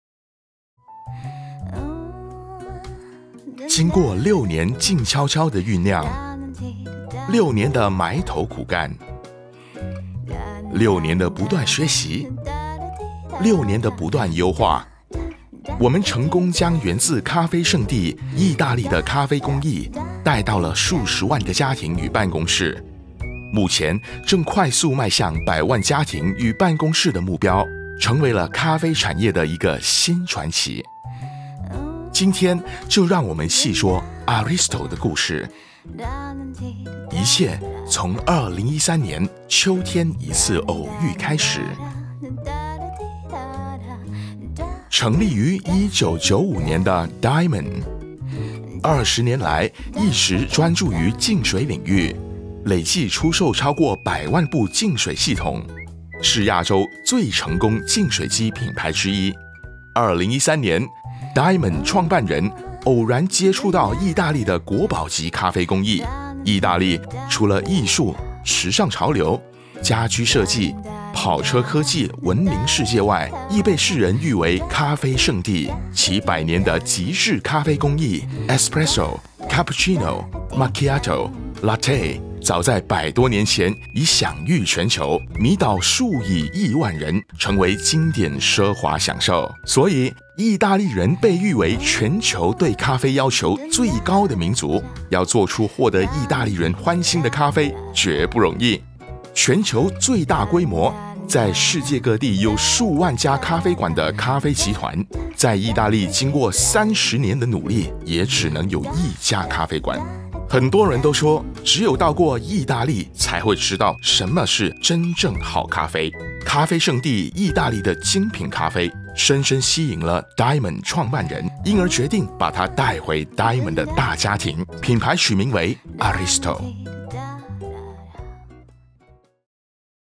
ARISSTO Coffee~ 来自咖啡圣地意大利的ARISSTO
意大利被誉为咖啡圣地，连全球最大咖啡连锁店亦不敢涉足。而意大利人，亦被视为世界上对咖啡最有要求的民族,意大利咖啡成为了全球咖啡专家学习与模仿的目标。

意大利人认为浮夸的花式只有短暂的惊喜，总不及隽永馥郁的味道。每一杯真正的意大利咖啡，都具有美丽的咖啡crema，至于Cappuccino，泡沫厚薄适中，并呈现出完美的三层咖啡，给予您多层次的咖啡享受。很多品尝过ARISSTO真正意大利咖啡的人，都会一试着迷，回味无穷！